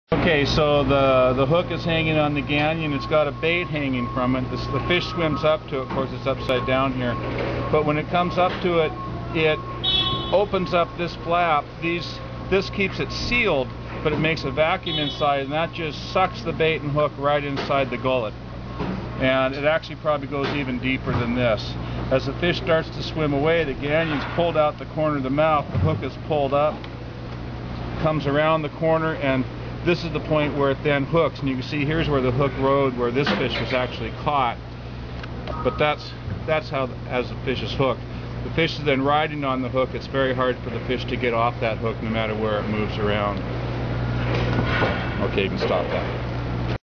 Pacific halibut Hooking - Location
Most Pacific halibut caught on circle hooks are hooked in the corner of the jaw.  Most of these Pacific halibut are hooked on the white side.  This is the result of taking the bait and then swimming up and away.  The gangion pulls along the corner of the white-side jaw, and the fish is hooked (or sometimes not).  Sometimes Pacific halibut are hooked in the corner of the dark-side jaw.  This is the result of the bait being suspended or for some reason not lying on the bottom. When the Pacific halibut takes this hook, it then swims down and away, with the gangion pulling out of the dark-side corner of the jaw, resulting in a dark-side hooking.

IPHC Staff